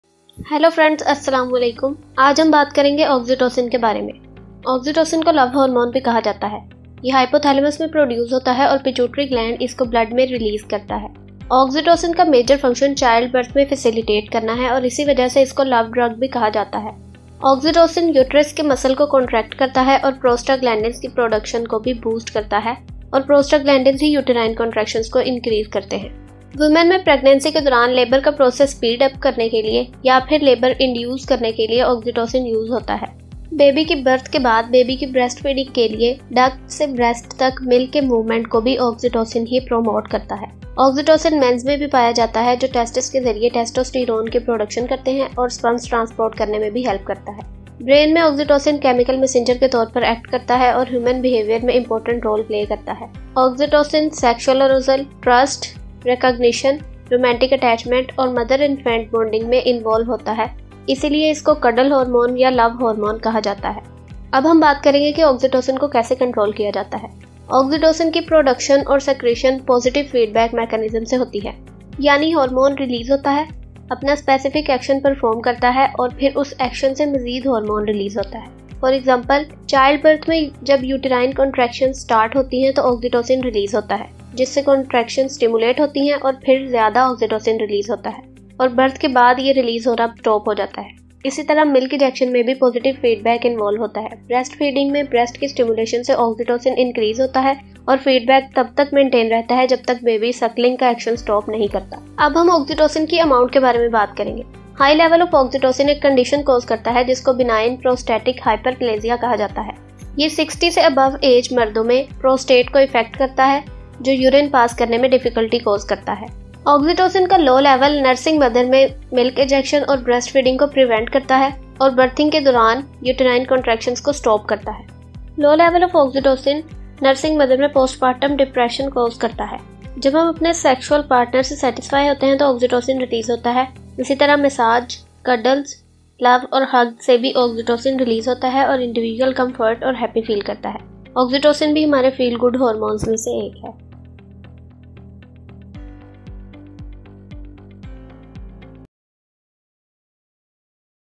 Oxytocin | The Love Hormone | endocrine system
Oxytocin | The Love Hormone | endocrine system | Hormone
oxytocin is also called love hormone. It involves in Child birth , Romance and breast feeding. In this video I explain it simply.